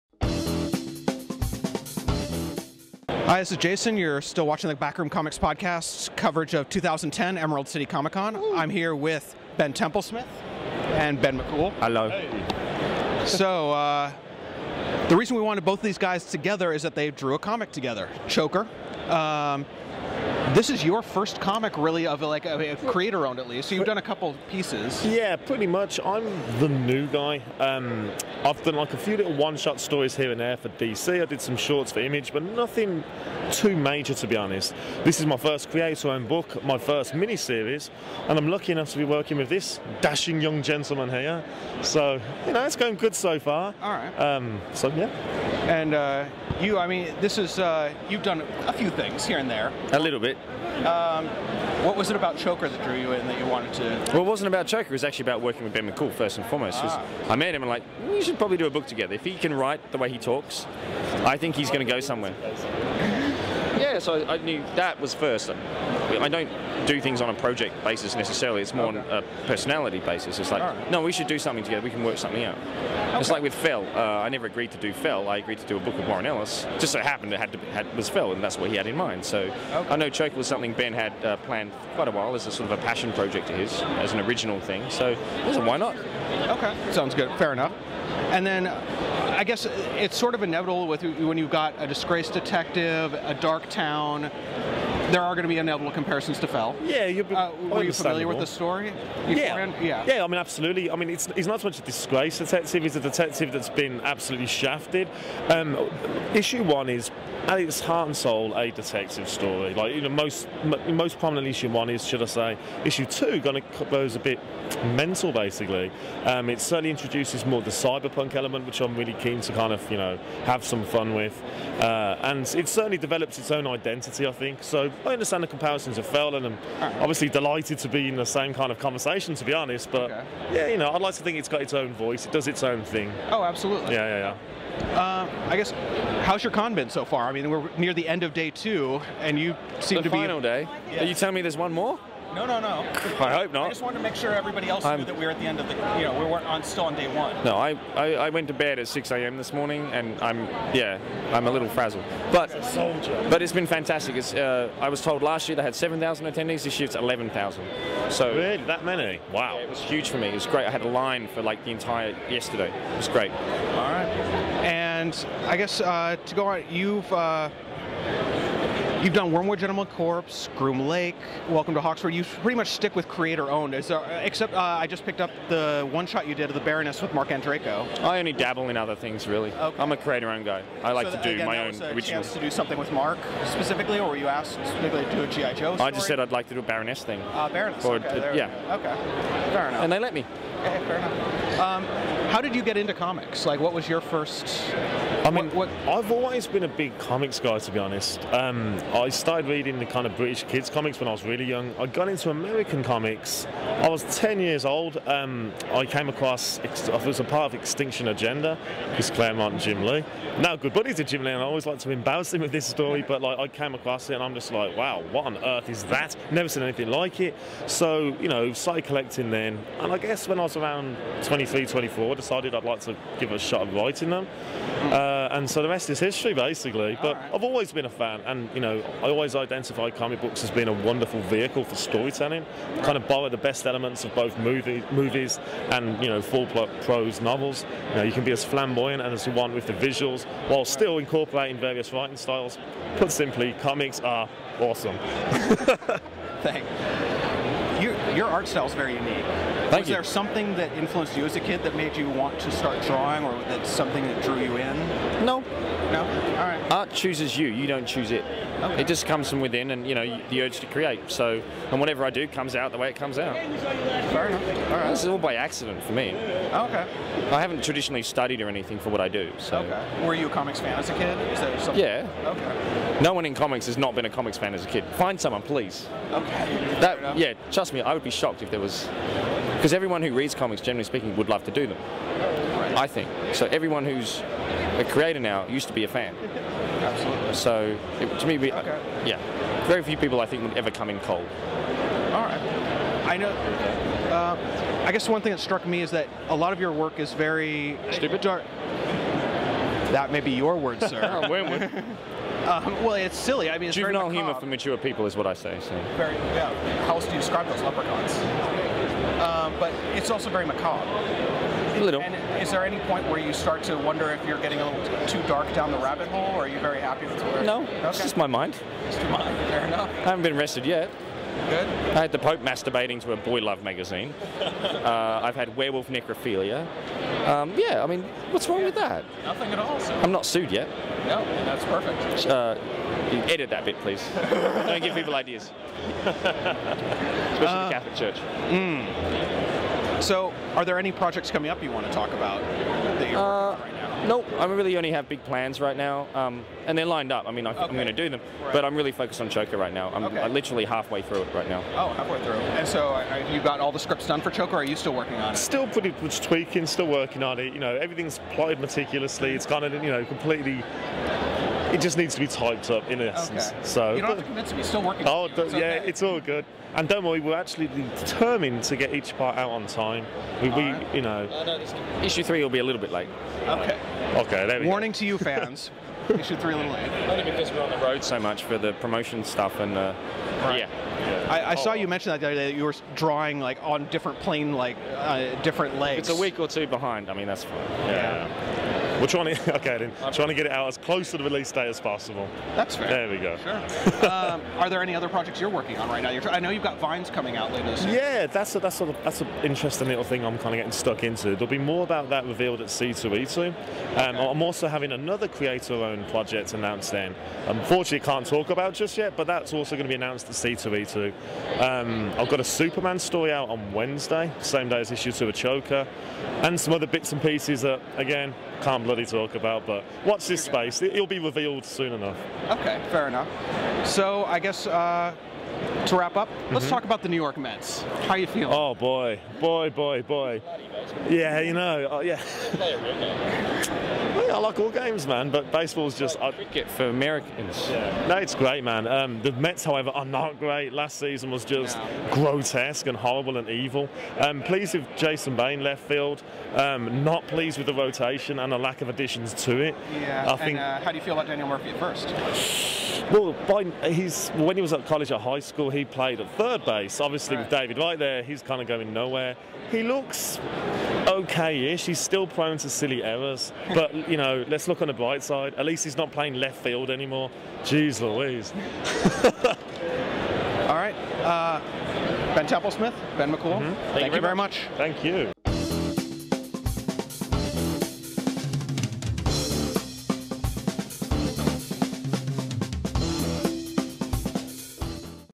Backroom Comics Podcast - Ben McCool & Ben Templesmith Interview at ECCC 2010
An interview with Ben McCool & Ben Templesmith, the writer and the artist on Choker, at the 2010 Emerald City ComiCon by the Backroom Comics Podcast.